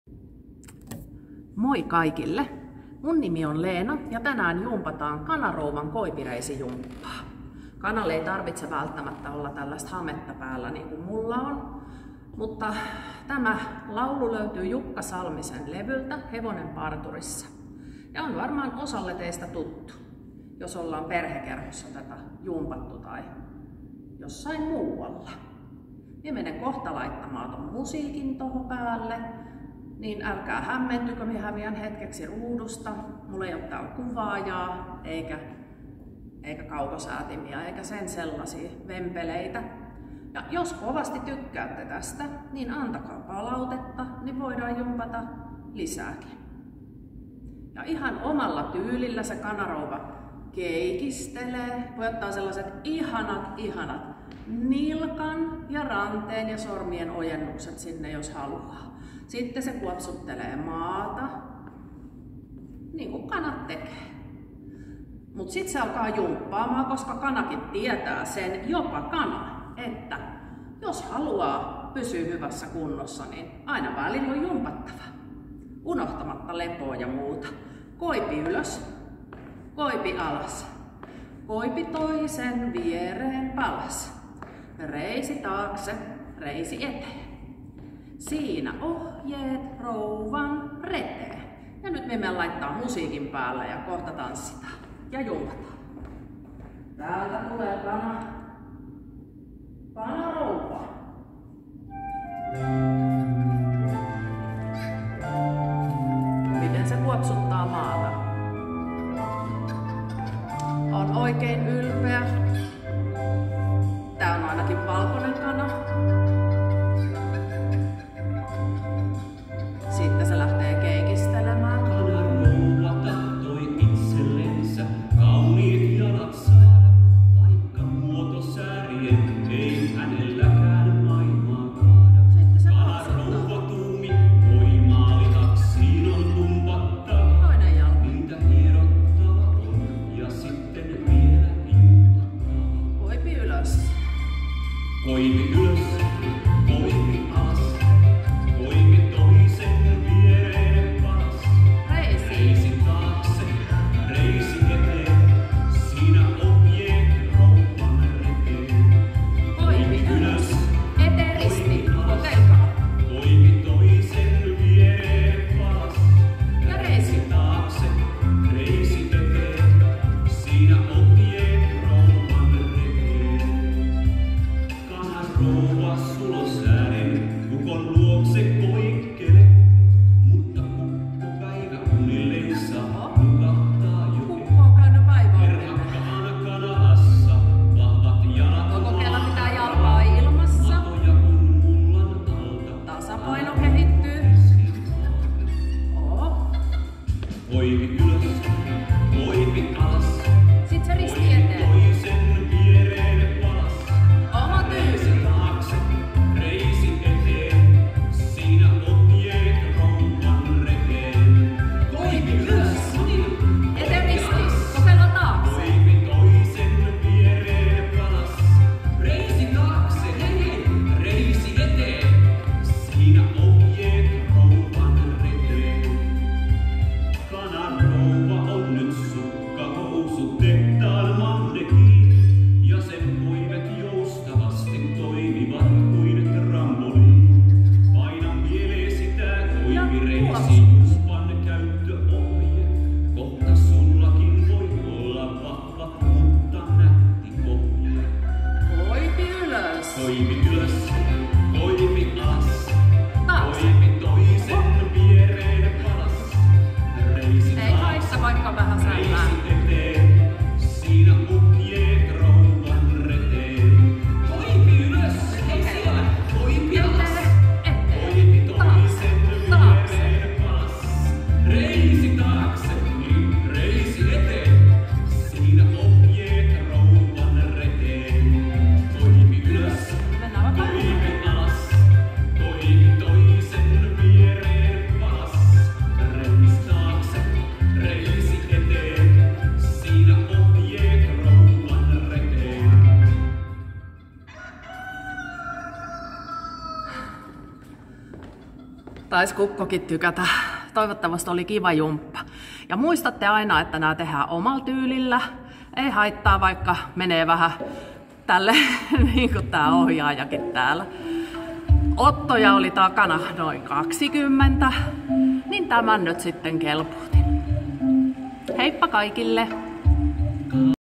Kanarouvan koipireisi-jumppa
Pääsiäisen lähestyessä Leena on tehnyt meille kivan Kanarouvan koipireisi-jumpan. Tule mukaan tanssimaan!